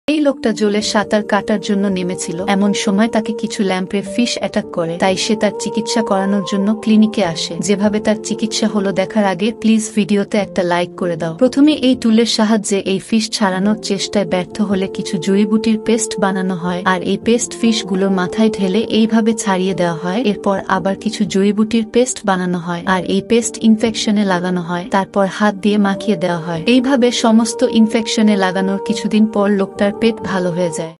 লোকটার পেটে এটা কি কামড়ে ধরেছে
ল্যাম্প্রেফিশ লোকটার পেটের কি অবস্থা করল